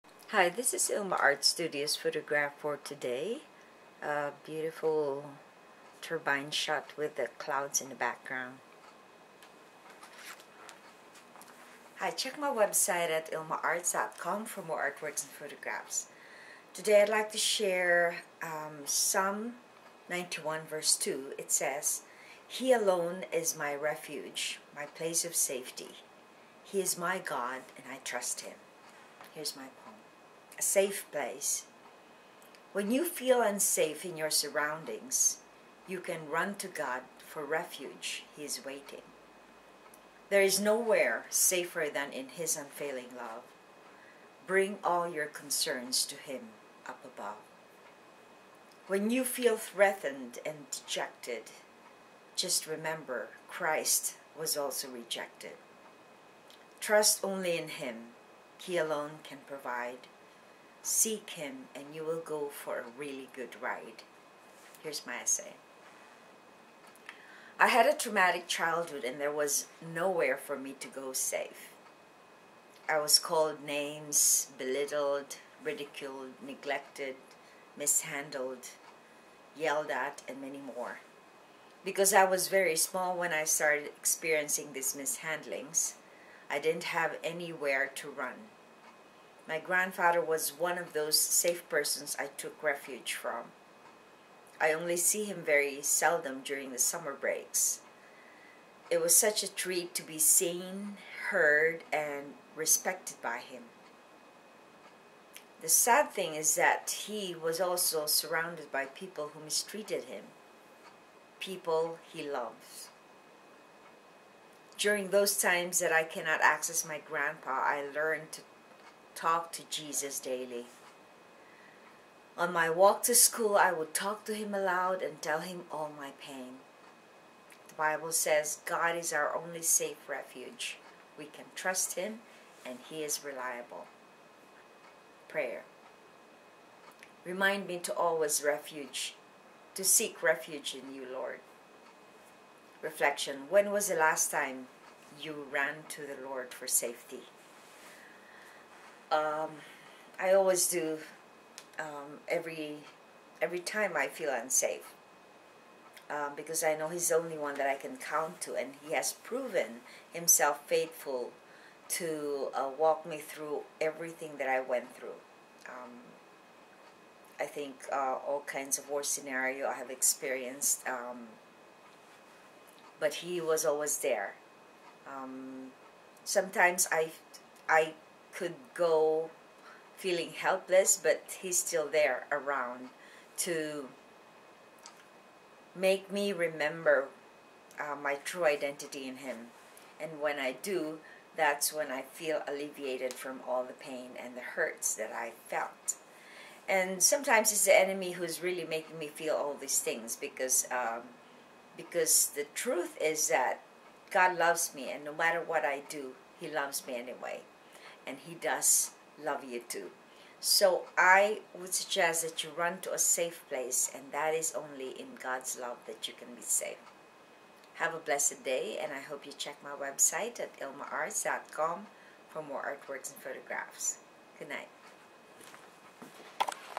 A SAFE PLACE poem devotional by ILMA
May 15
He alone is my refuge, my place of safety; he is my God and I trust him. -Psalm 91:2(NLT)
A SAFE PLACE
A poem and essay by ILMA
When you feel unsafe in your surroundings
You can run to God for refuge, he is waiting
There is nowhere safer than in his unfailing love
Bring all your concerns to him up above.
When you feel threatened and dejected
Just remember Christ was also rejected
Trust only in him, he alone can provide
Seek him and you will go for a really good ride.
I had a traumatic childhood and there was nowhere for me to go safe. I was called names, belittled, ridiculed, neglected, mishandled, yelled at, and many more. Because I was very small when I started experiencing these mishandlings, I didn’t have anywhere to run. My grandfather was one of those safe persons I took refuge from. I only see him very seldom during the summer breaks. It was such a treat to be seen, heard and respected by him. The sad thing is that he was also surrounded by people who mistreated him, people he loves. During those times that I cannot access my grandpa, I learned to talk to Jesus daily. On my walk to school, I would talk to him aloud and tell him all my pain. The bible says, God is our only safe refuge. We can trust him and he is reliable.
PRAYER
Remind me to always seek refuge in you Lord.
REFLECTION
When was the last time you ran to the Lord for safety?
For more testimonials about God’s word, please go to YouTube and type ILMA’s poem devotional or go posts.